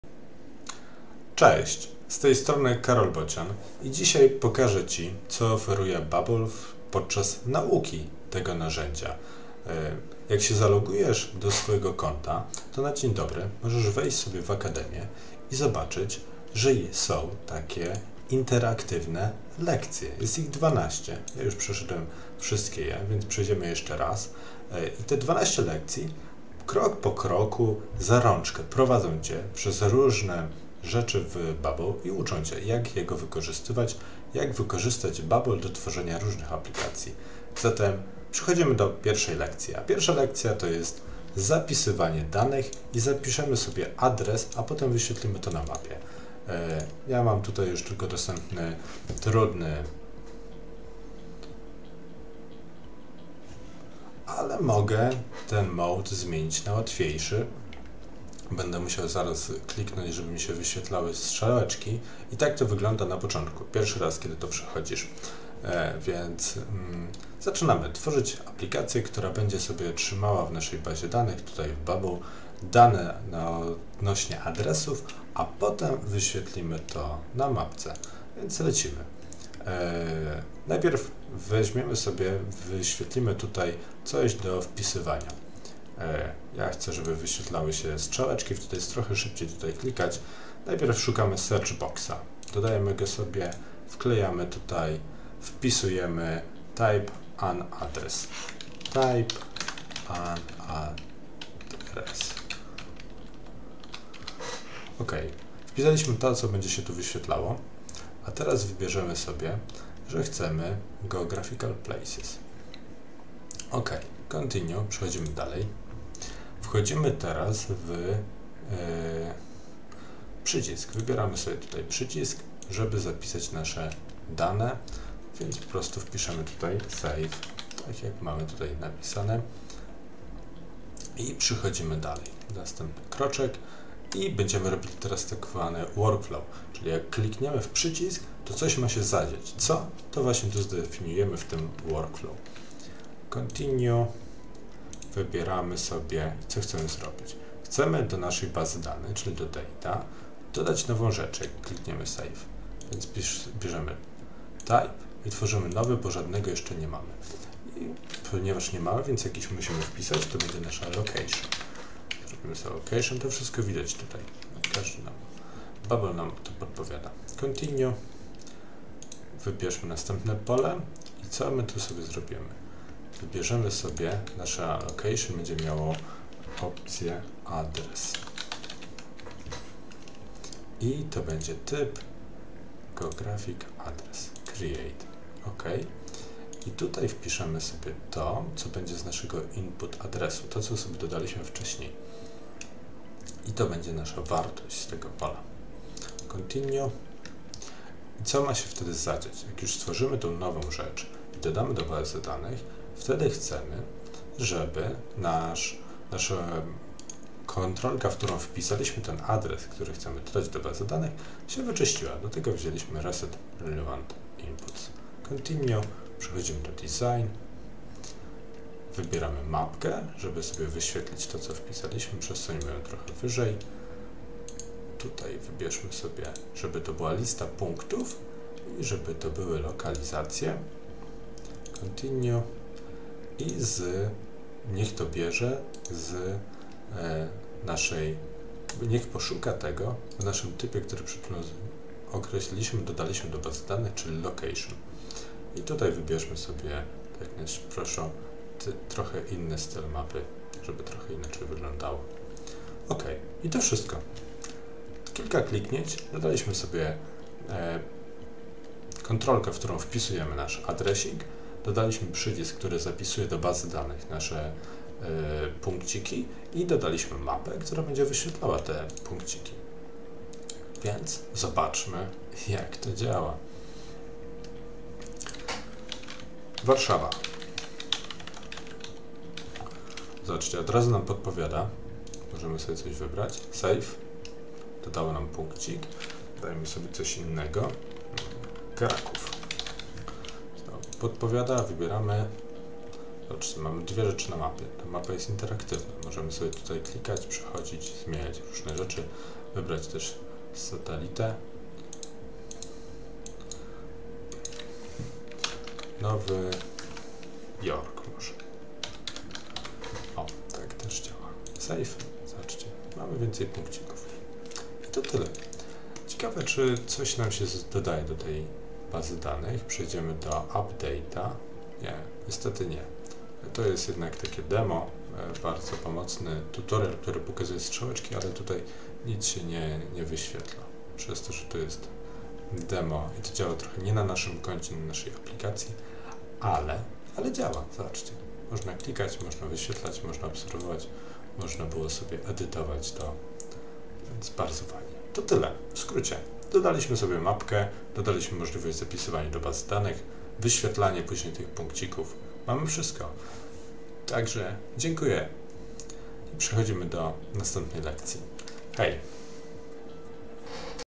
Interaktywna Nauka - Jak zapisać dane i wyświetlić na mapie? - tutorial w Bubble.io (1)
Cześć!

W tym filmie przedstawiam:
Jak zapisać dane w Bubble.io do bazy danych (adresy), a potem wyświetlić na mapie?
Zobacz pierwszą lekcję interaktywnego tutoriala przygotowanego przez Bubble.io!

Saving data
Save addresses and display them on a map